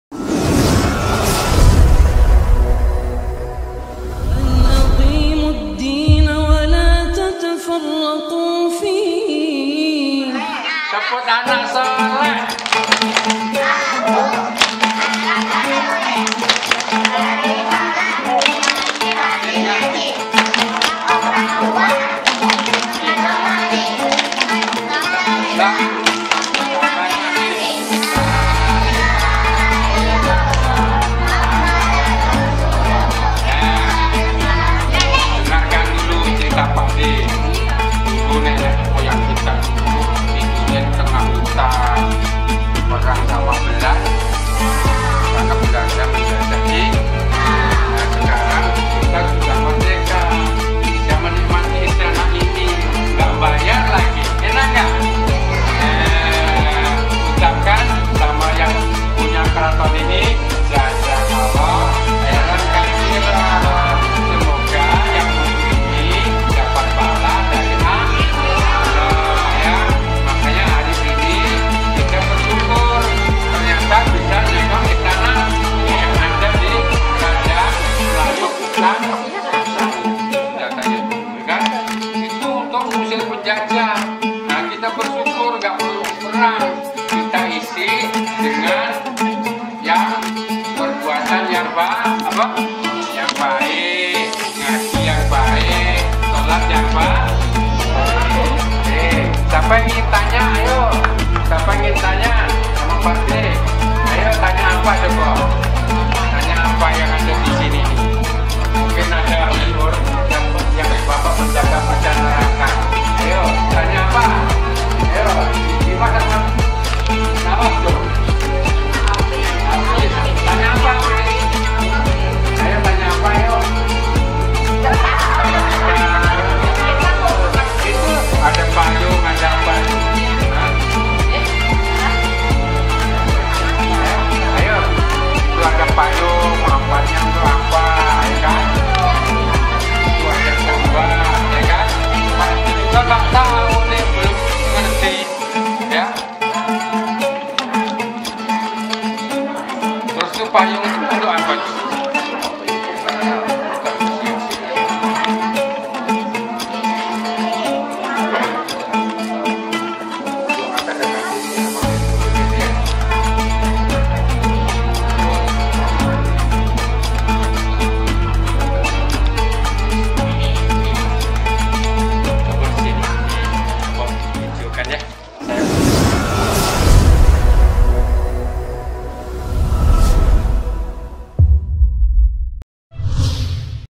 PAUD Al- Fatah Kembayan kunjangan diKeraton Surya Negara Sanggau - KABAR KITA
Sebuah progam edukasi pendidikan dilingkungan PAUD Al- Fatah Kembayan